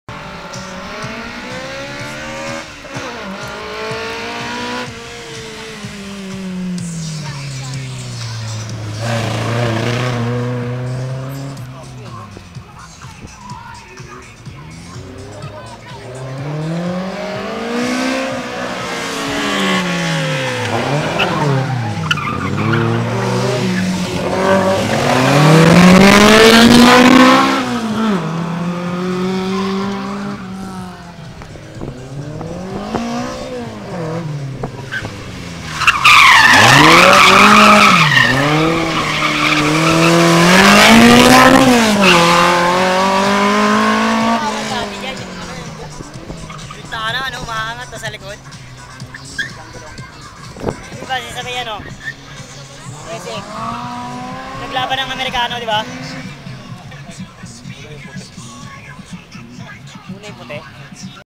Philippine Autocross Championship Series (2012 Finals)
(!!!Best played at 720p settings, located at the right part of the YouTube player--look for the Cog icon and choose 720p)
The final leg for 2012 season (slalom racing)
Held at the Bonifacio Global City on January 20,2013

Toyota Starlet vs. Toyota Starlet